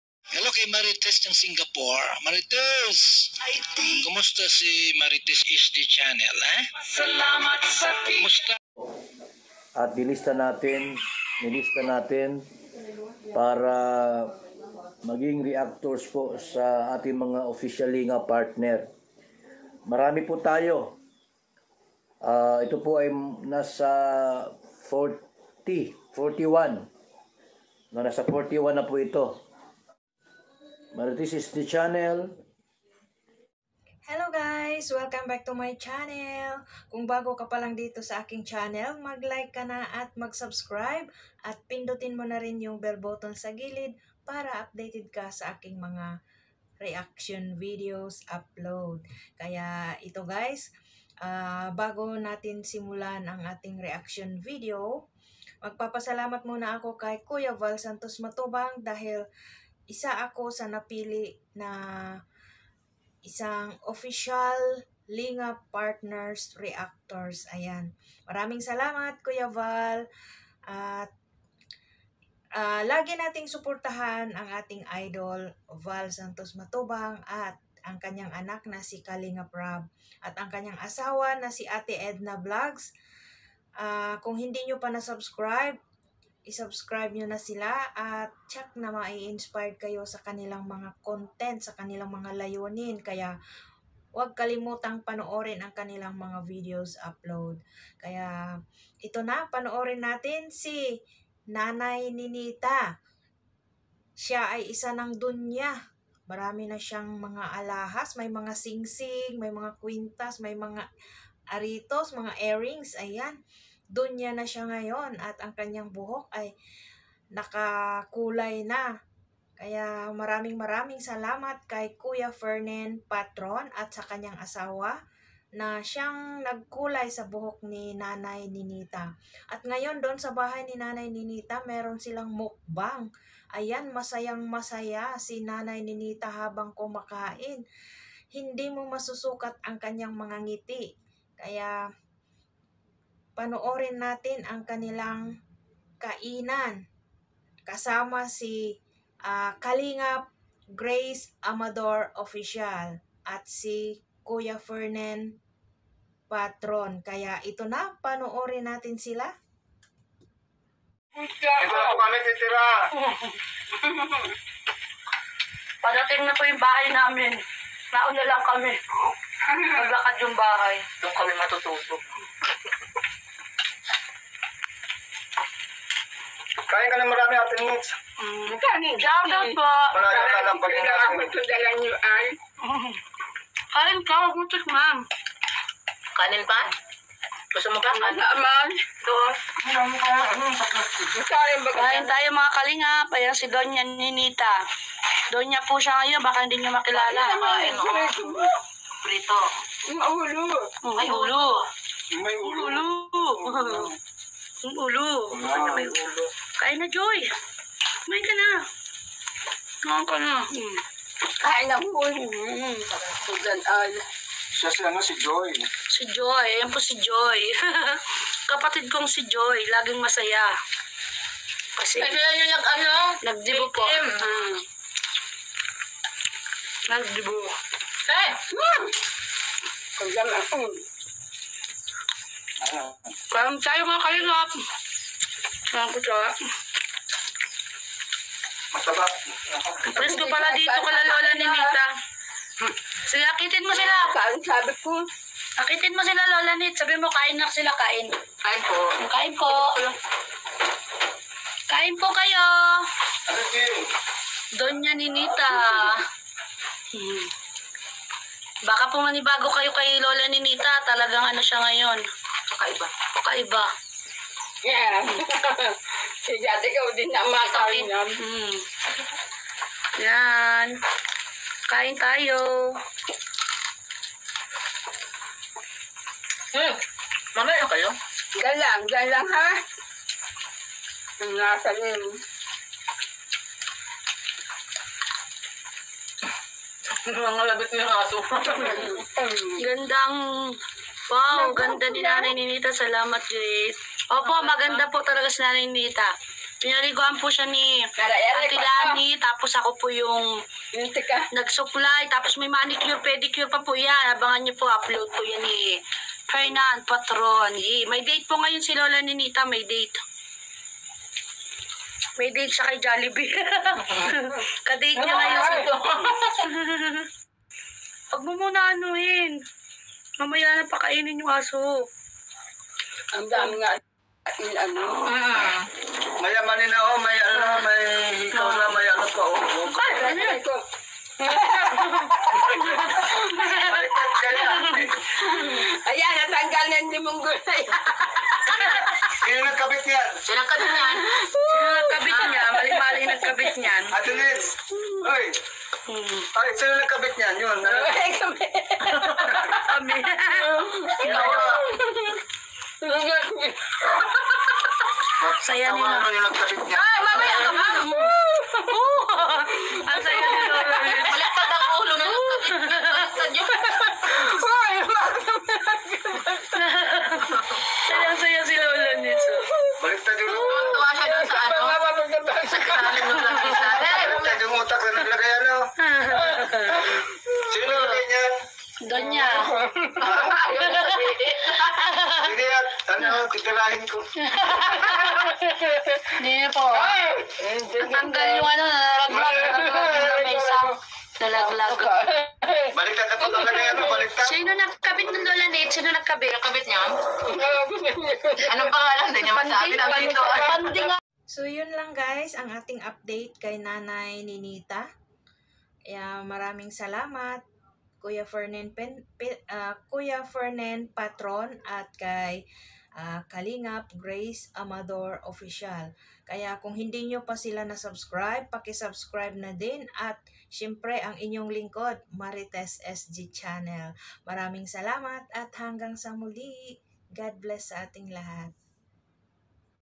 Sobrang saya ni Donya Nanay Nenita /Mukbang kasama
Masayang kumakain c Donya Nanay Nenita kasama c kuya Fernan Patron Kalingap Grace Amador Official at ate Lanie
Panoorin po natin may bagong Damit aritos singsing at kwentas